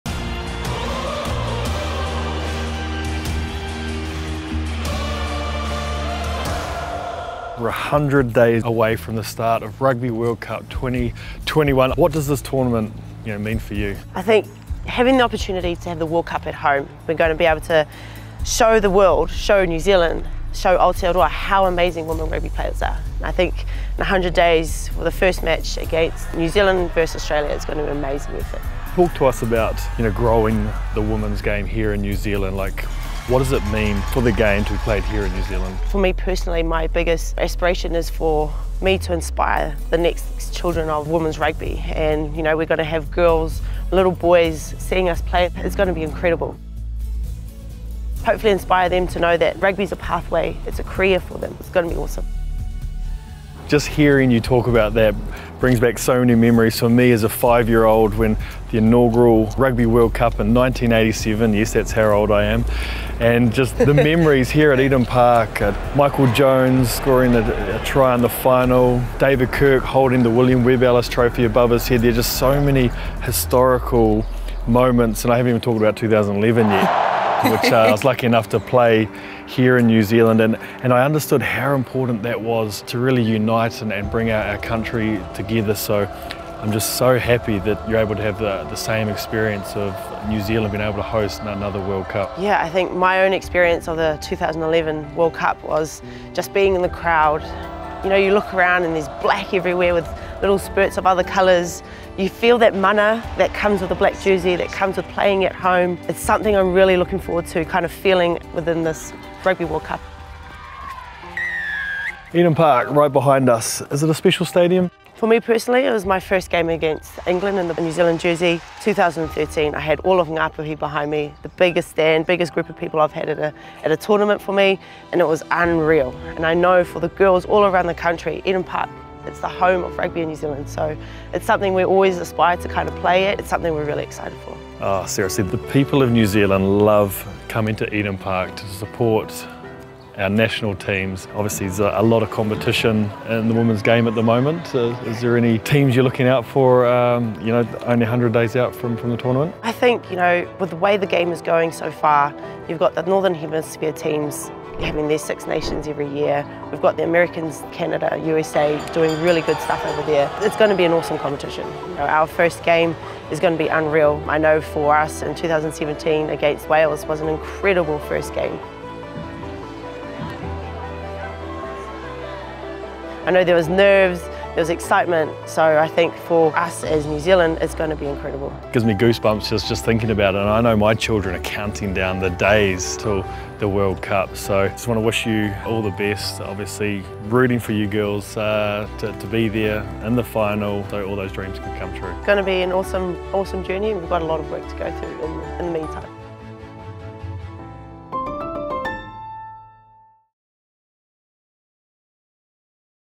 Dan Carter and Portia Woodman take a trip to Eden Park: Rugby World Cup 2021 Preview | Ep. 1
With 100 days to go until the Rugby World Cup 2021, Dan Carter and Portia Woodman take a trip to Eden Park with Mastercard to talk all things rugby!